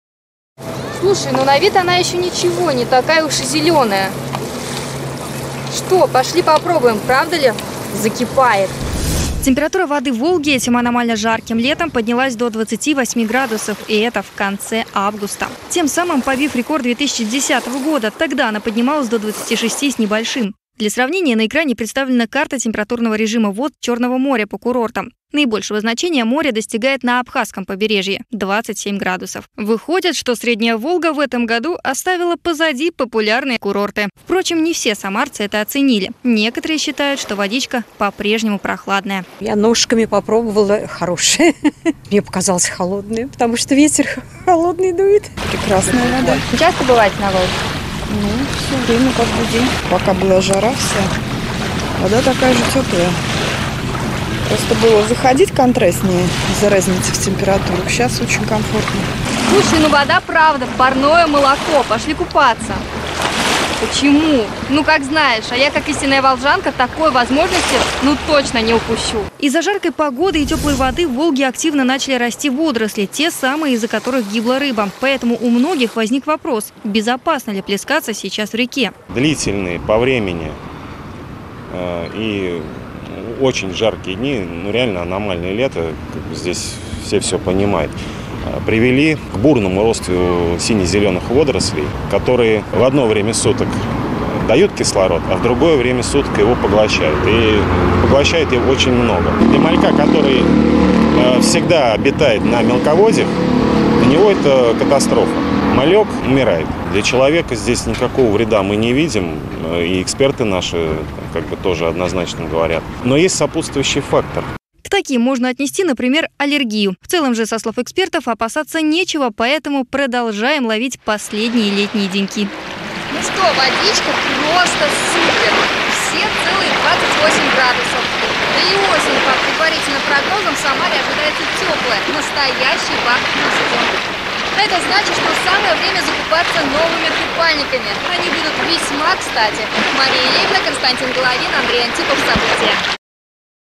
Температура воды в Волге этим аномально жарким летом поднялась до 28 градусов
Погода в Самаре продолжает бить все температурные рекорды. За лето вода в Волге прогрелась до 28 градусов, это самое большое значение за последние 30 лет! Что привело к активному росту водорослей. Безопасно ли нежиться в такой теплой, но зеленой воде человеку, - Выясняла Мария Левина.